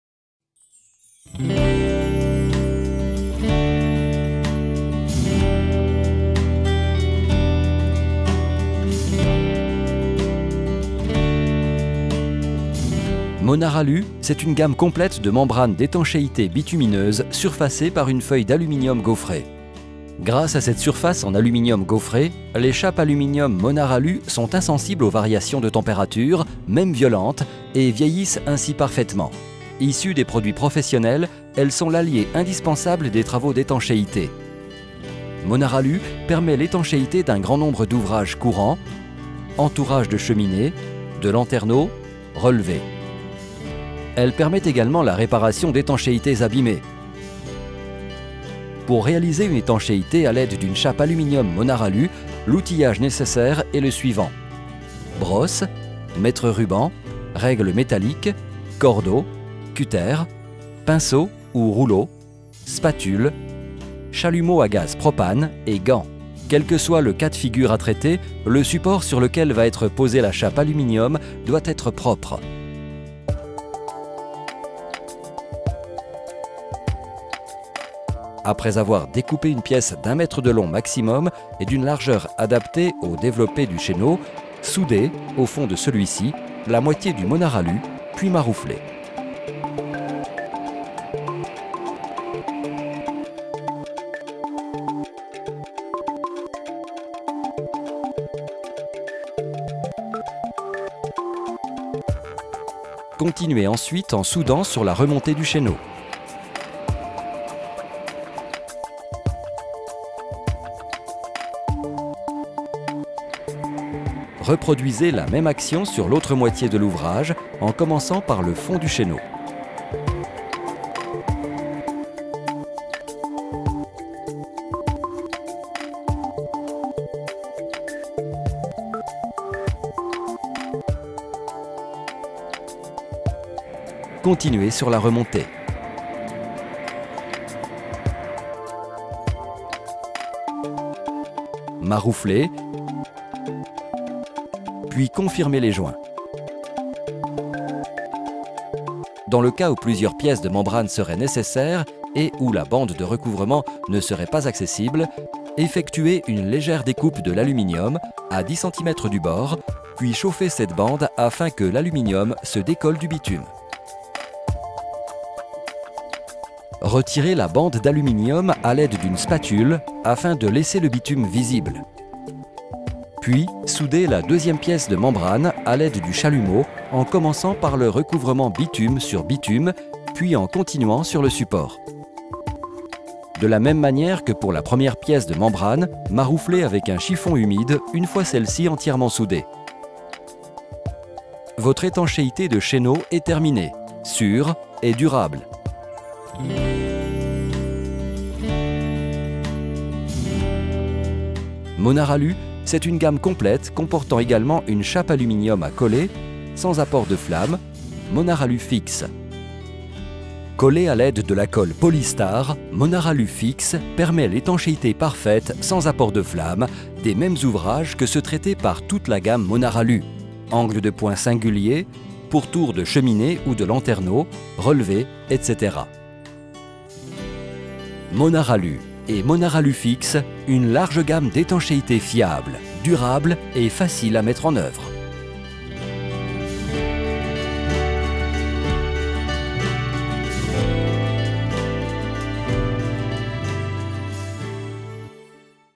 Monaralu - étancher un chéneau
Monaralu - l'indispensable de l'étanchéité

Monaralu, c'est une gamme complète de membranes d'étanchéité bitumineuses surfacées par une feuille d'aluminium gaufrée. Grâce à cette surface en aluminium gaufrée, les chapes d'aluminium Monaralu sont insensibles aux variations de températures et vieillissent ainsi parfaitement.